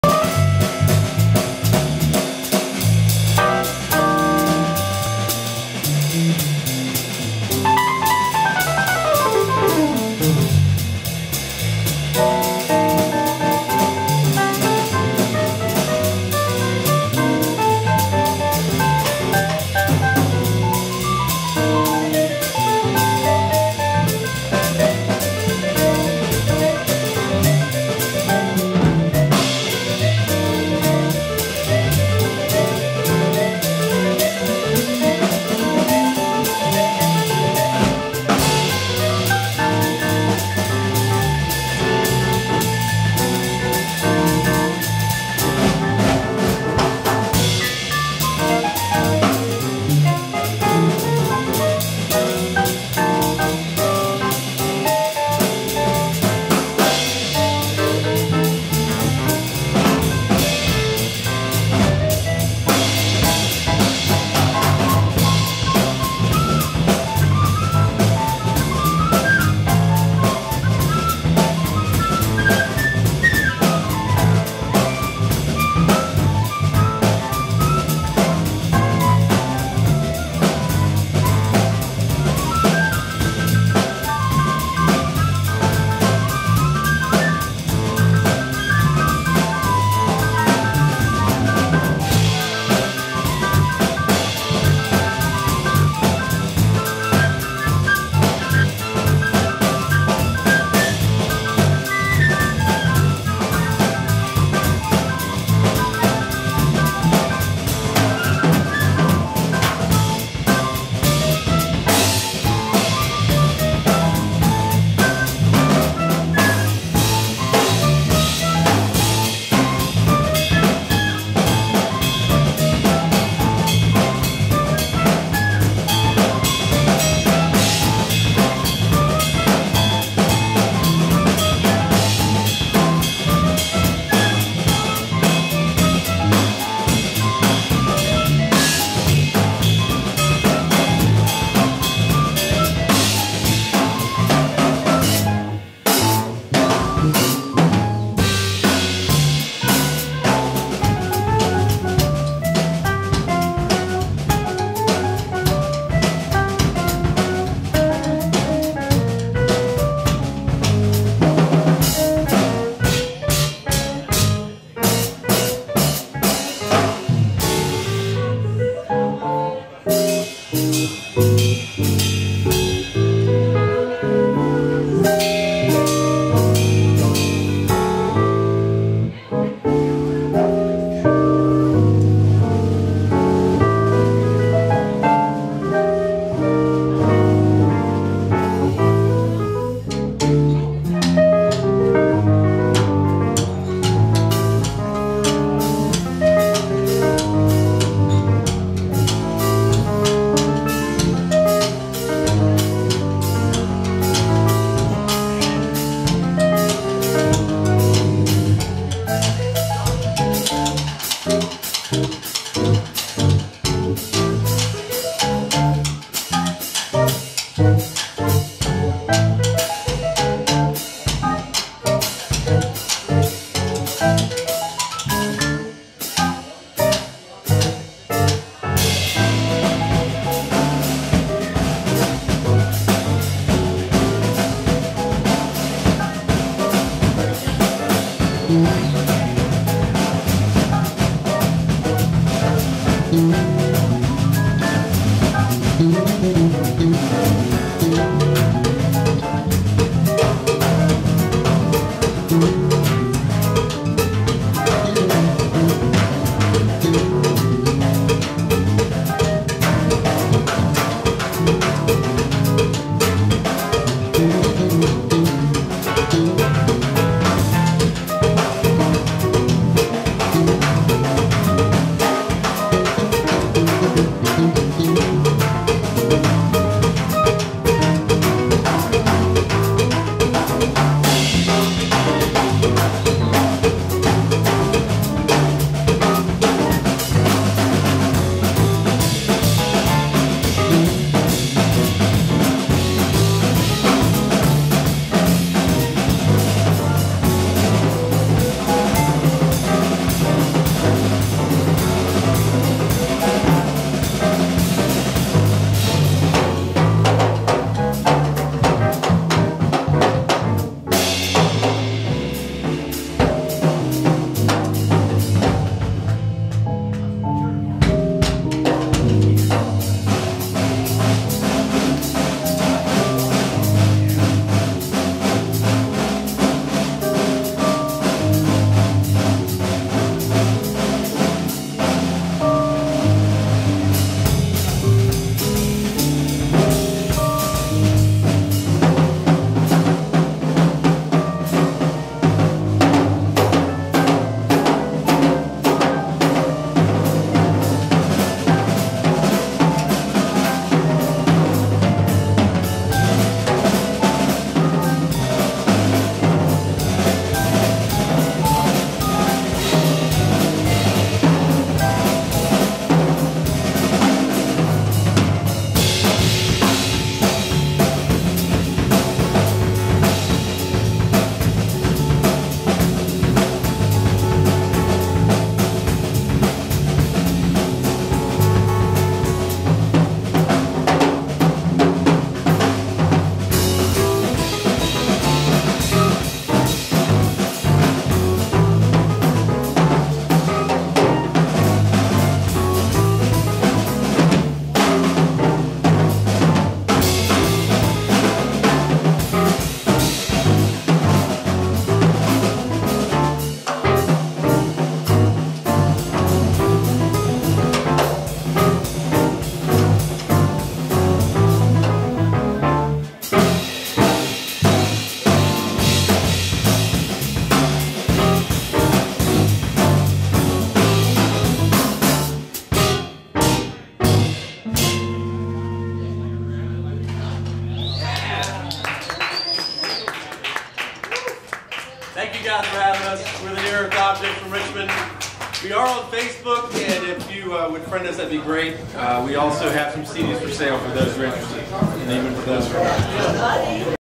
Near Earth Objects, with Gina Sobel, at Fellini's #9, Charlottesville
Joey Ciucci (keyboards), Nathan Goodwyn (bass), Scott Milstead (drums), Gina Sobel (flute).   This was their final number of the night -- and a great night it was!